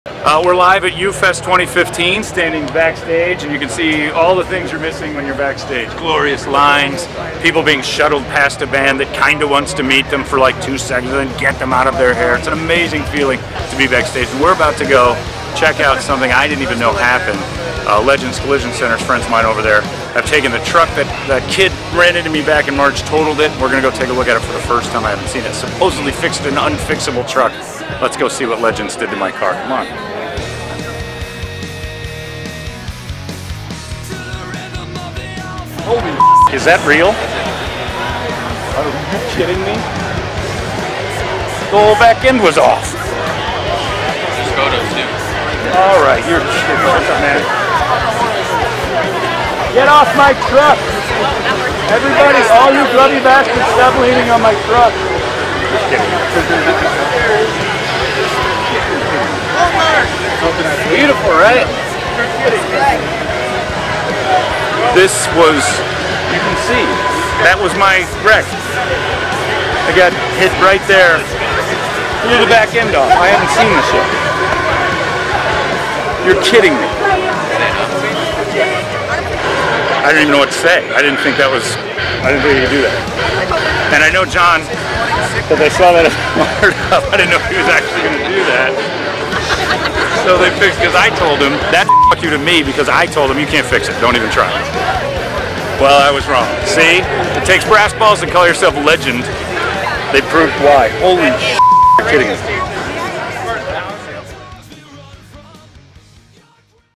98KUPD's John Holmberg Sees His Truck For The 1st Time After His Accident
98KUPD's John Holmberg Sees His Truck For The 1st Time After His Accident. Thanks to the gents over at Legends Collision Center for fixing Holmberg's truck!